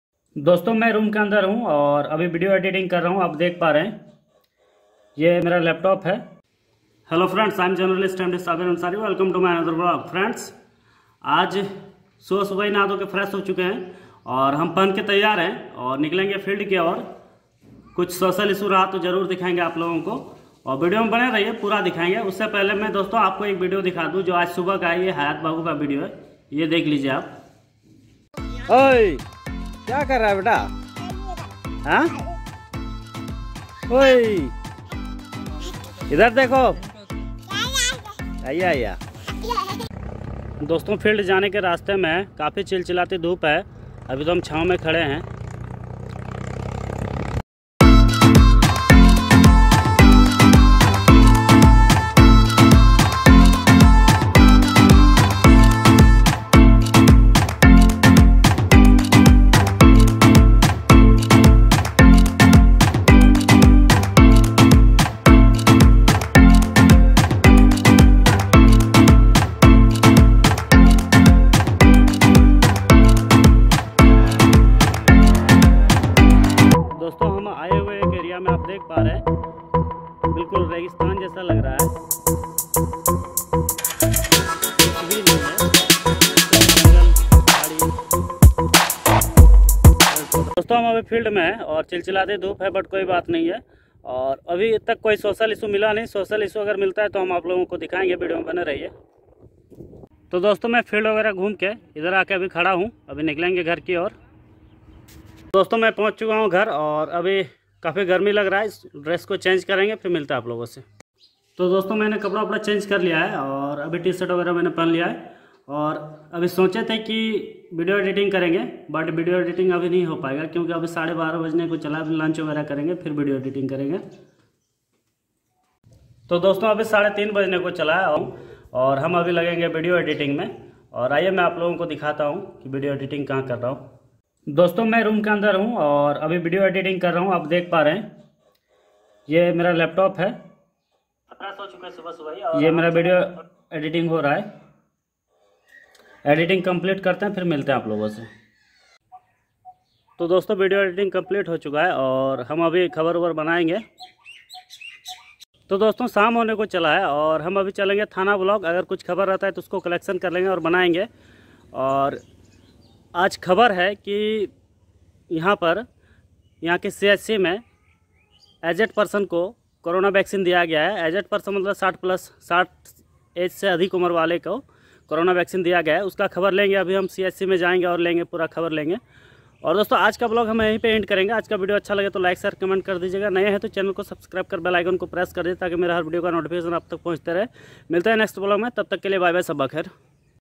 HAYAT ANSARI KA NEW STYLE
is video me hayat ansari ka ek stayle dikahya gya hai mere dwra feild ghute hue  dikhaya gya hai ek jungle area ko dikahya gya hai kyi road ko dikahya gya hai.